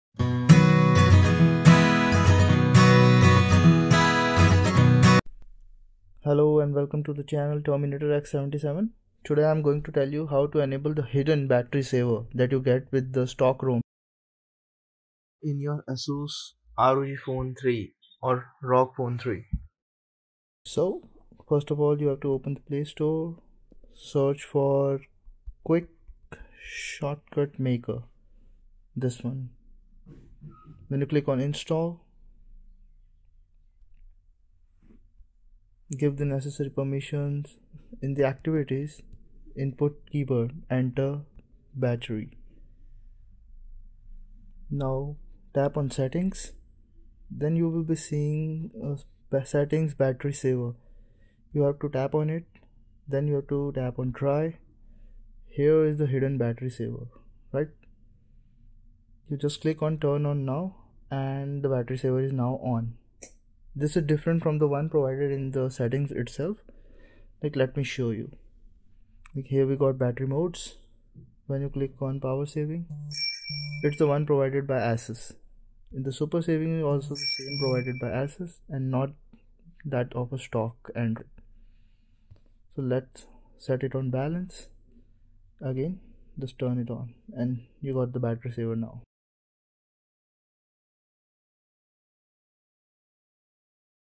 Asus ROG Phone 3 extended battery saver enable | Improve battery life in ROG phone 3
Enable extended battery saver in ROG phone 3 without enabling developer options.

Please see the full video it is legit and no click-bait